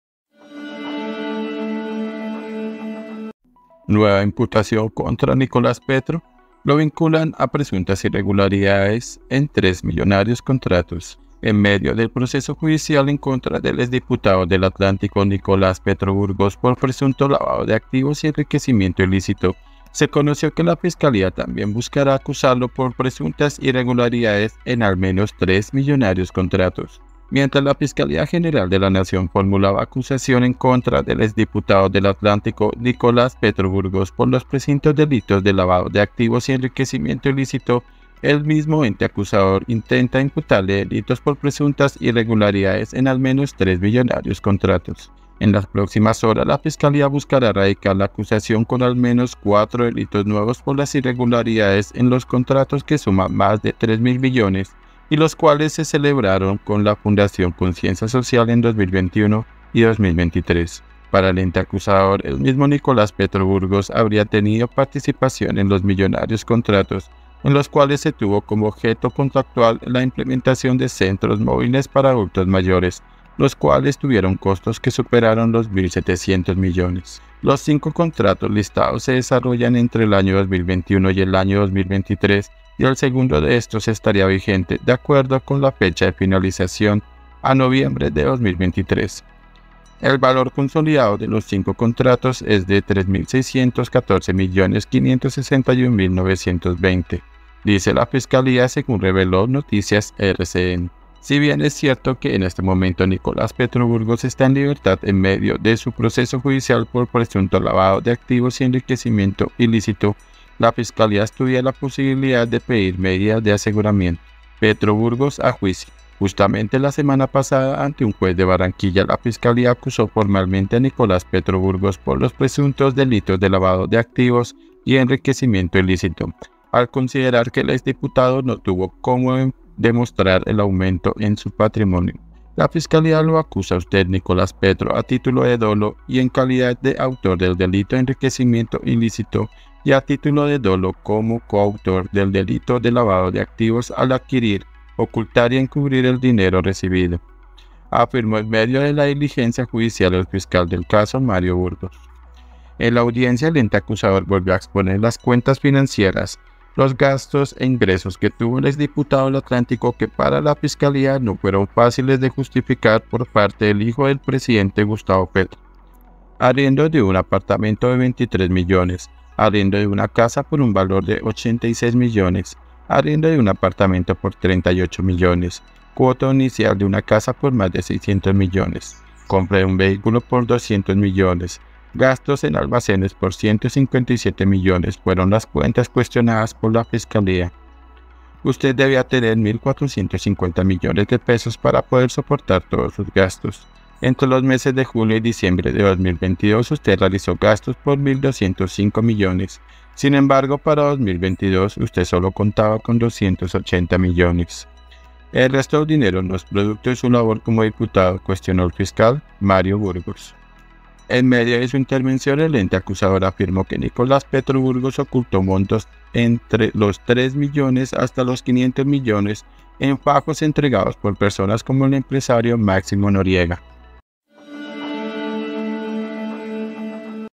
ÚLTIMA HORA 🛑 SE VIENE NUEVA IMPUTACIÓN DE CARGOS CONTRA NICOLAS PETRO
En medio del proceso judicial en contra del exdiputado del Atlántico, Nicolás Petro Burgos, por presunto lavado de activos y enriquecimiento ilícito, se conoció que la Fiscalía también buscará acusarlo por presuntas irregularidades en al menos tres millonarios contratos.
VANGUARDIA